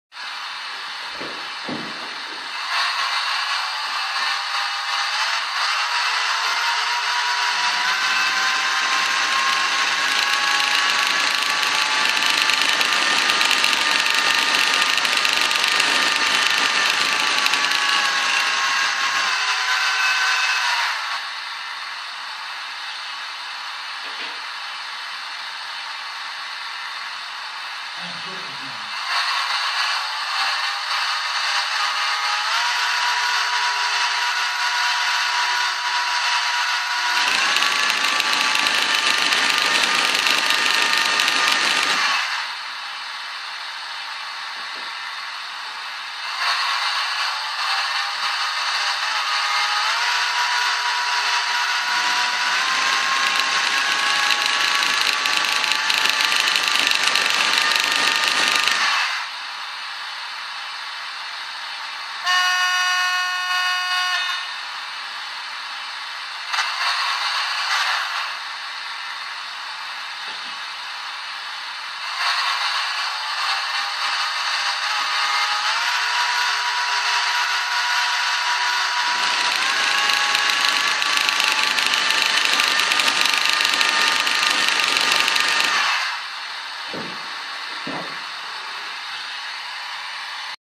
H0 mkb V 19 mit Chemiezug
Szwne aus Liebenau nachgestellt